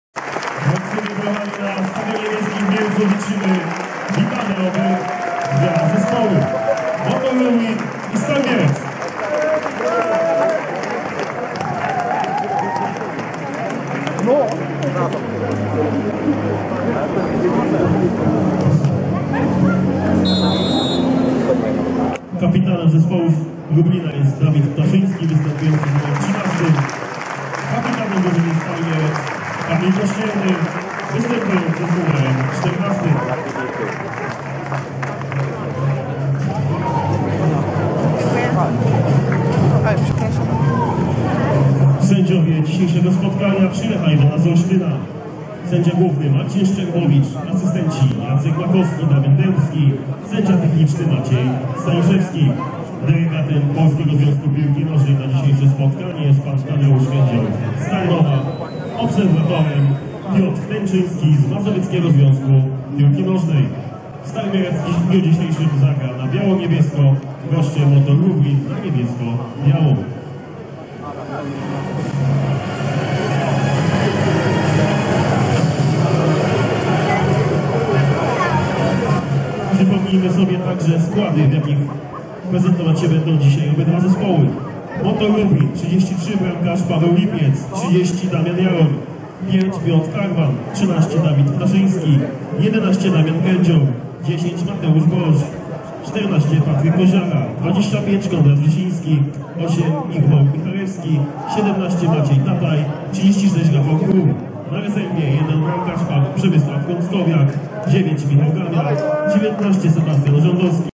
Stal Mielec - Motor Lublin początek meczu
27 października w Mielcu odbył się mecz 14. kolejki II ligi piłkarskiej. Stal Mielec podejmowała Motor Lublin. Mielczanie pewnie wykorzystali kilka przeprowadzonych akcji i pokonali rywala 3:1  Na meczu zabrakło kibiców przyjezdnej drużyny, decyzje o zakazie ich przyjazdu wydał Wojewoda Rzeszowski. Taka decyzja nie spodobała się też mieleckim kibicom z "młyna", którzy na znak protestu nie dopingowali swoich piłkarzy.